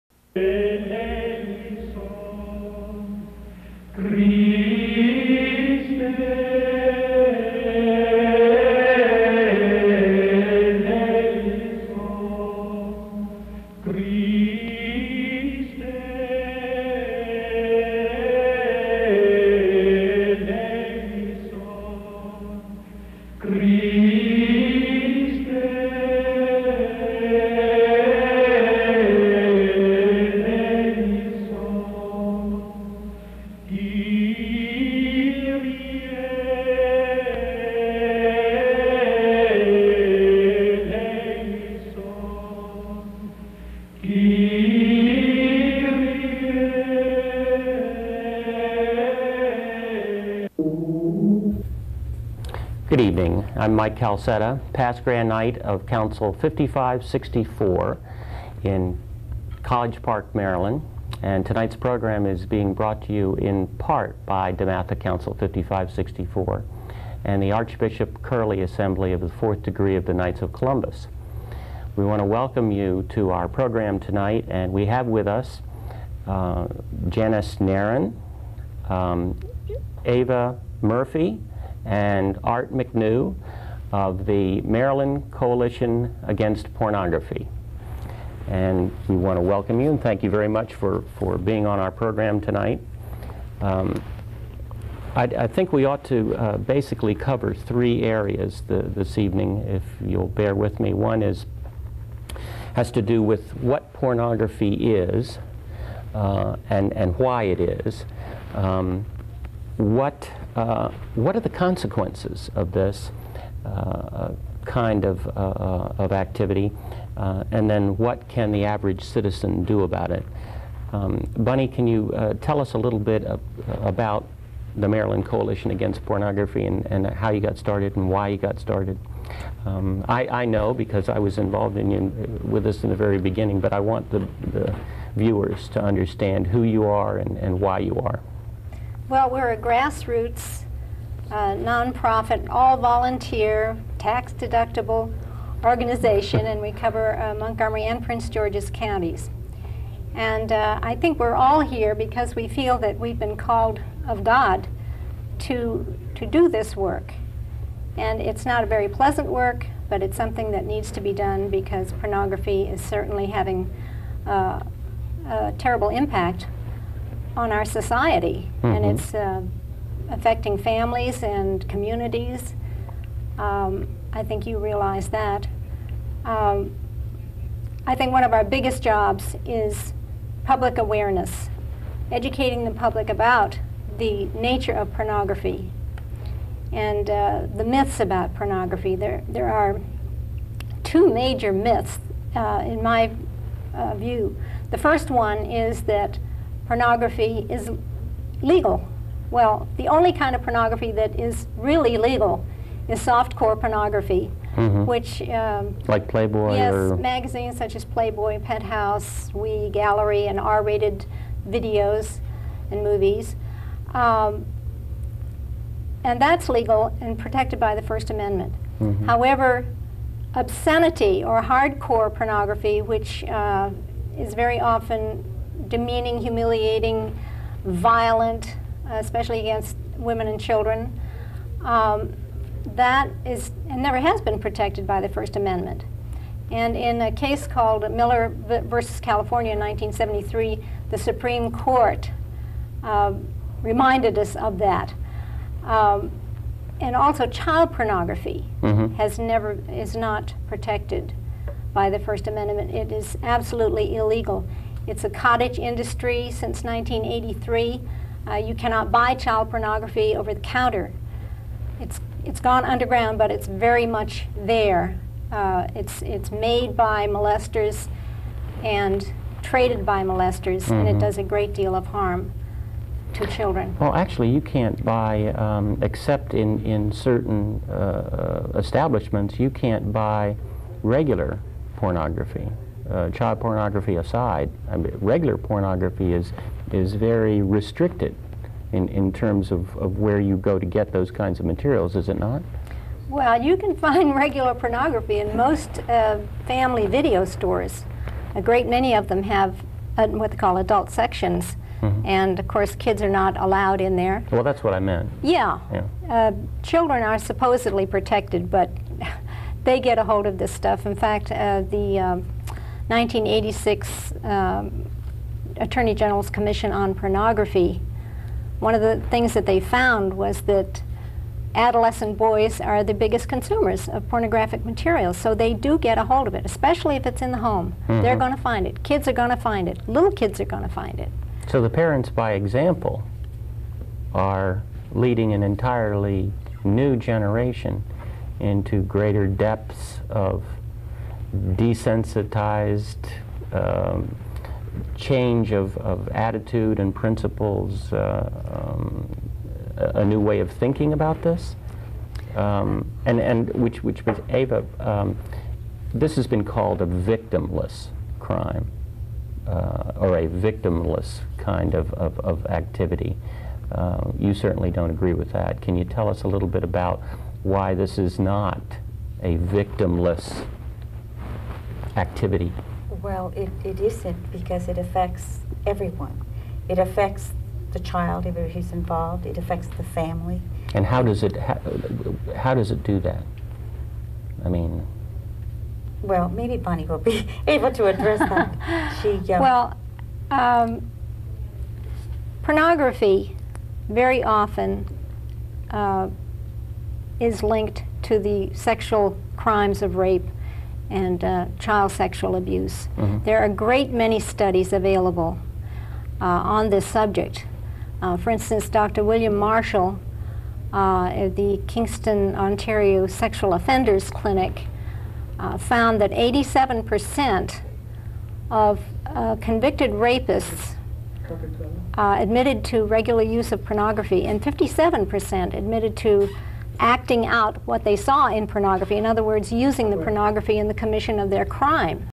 Pornography in the Community I
Janice Nairn, Eva Murphy, President and Vice President of the Maryland Coalition against Pornography and Arthur McKnew , member of the Knights of Columbus and of the Maryland Coalition against Pornography were interviewed by Michael Calsetta about their efforts to reduce and control the use of pornogarphy in Prince Georges County and Maryland.